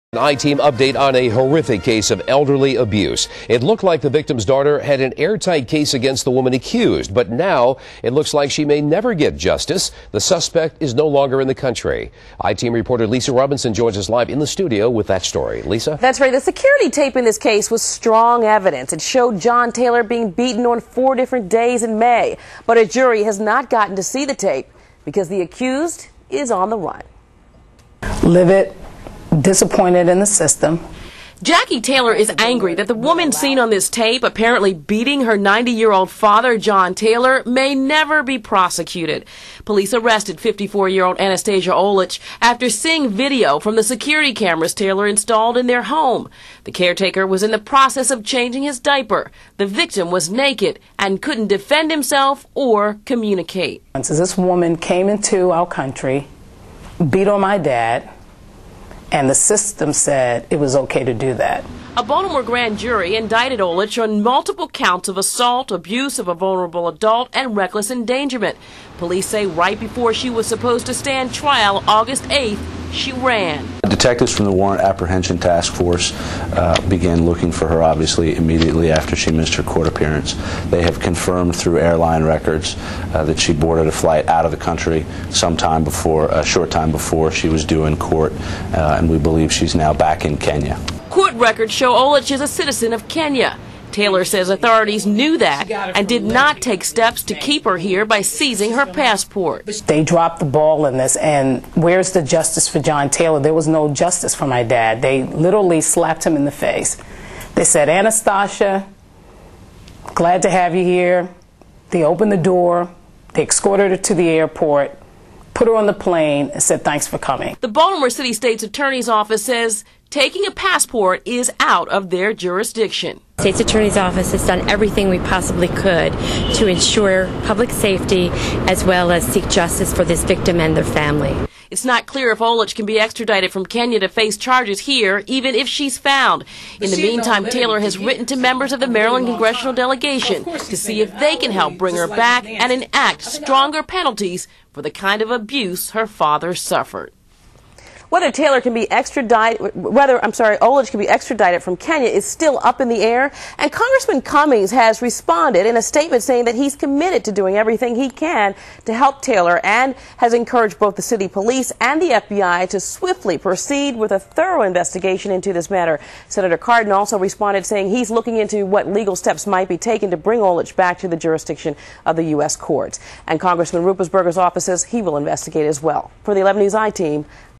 Woman Accused In Videotaped Beating Flees Country
The woman who caught her elderly father's caretaker abusing him on videotape may never get justice because the accused woman fled the country.